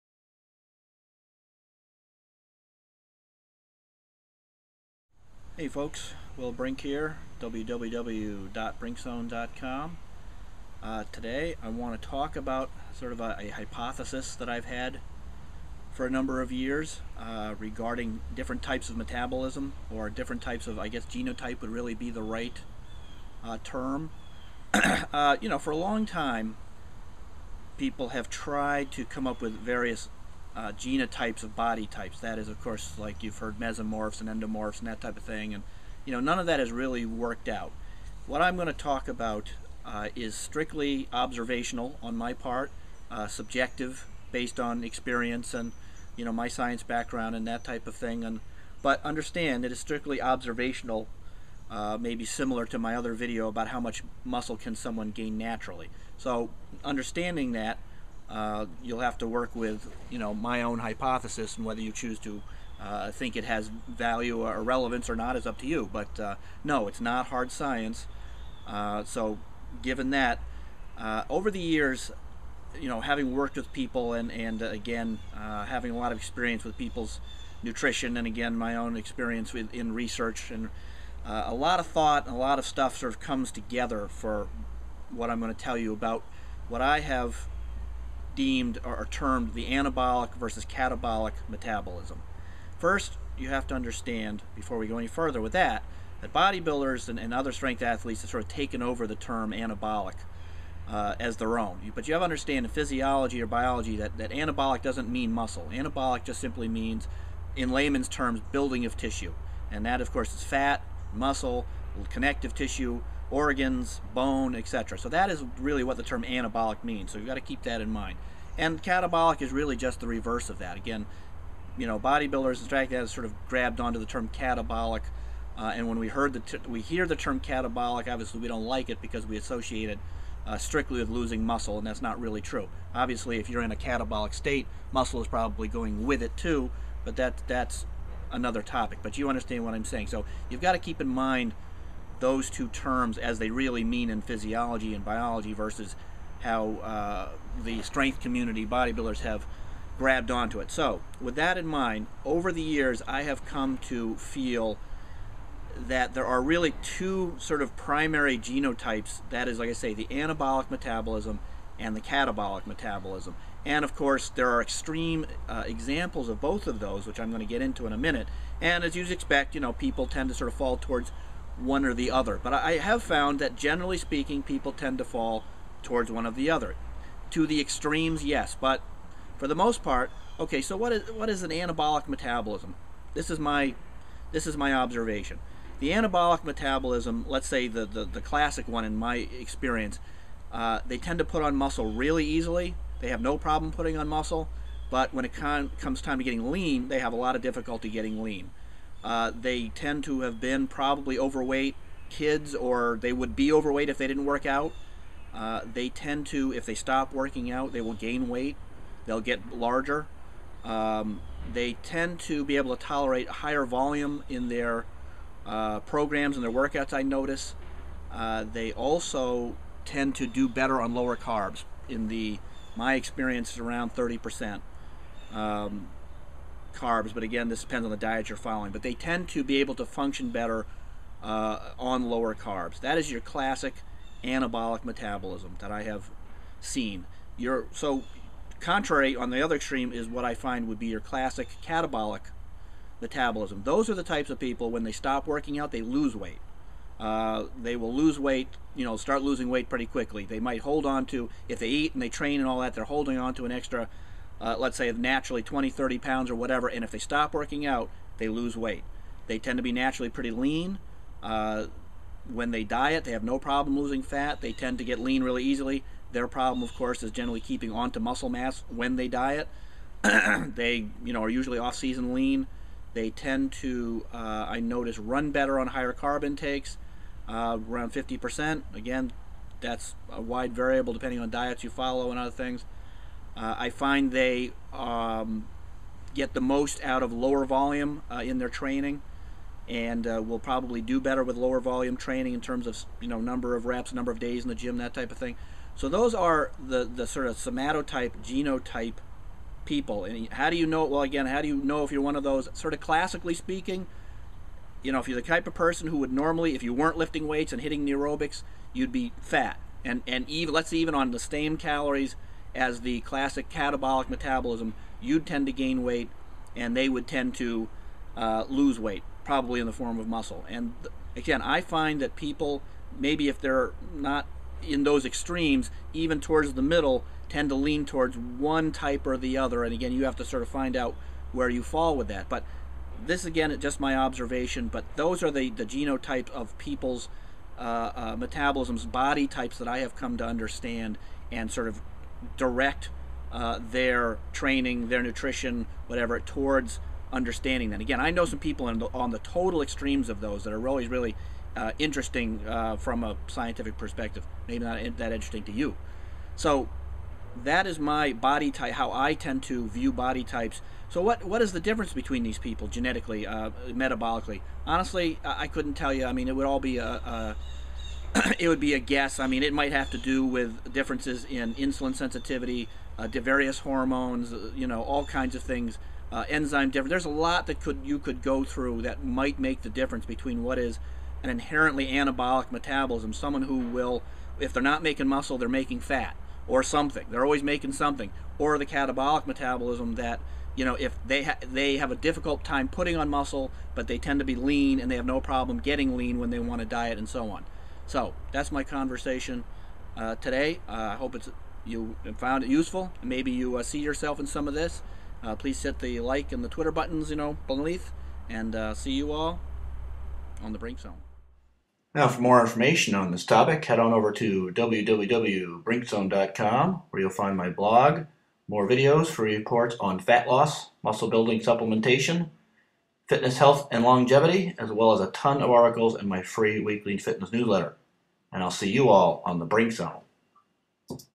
Anabolic vs Catabolic Metabolism
Anabolic Metabolism vs Catabolic Metabolism. Do you have an anabolic or catabolic metabolism? What's the difference? How does one know, and how do they train and eat for one type ve. the other? Learn all that in this vid!!!